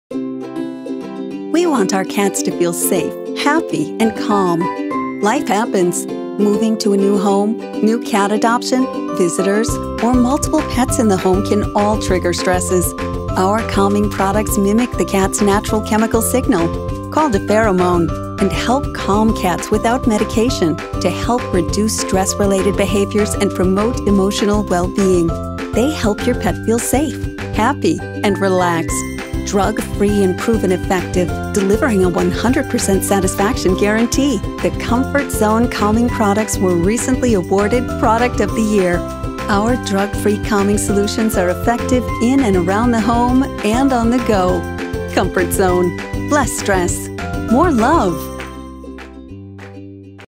Life Happens: Comfort Zone Products Can Help Reduce Unwanted Cat Behaviors
Our calming products mimic the cats natural calming signal called a pheromone and help calm cats without medication to help reduce stress related behaviors and promote emotional well being.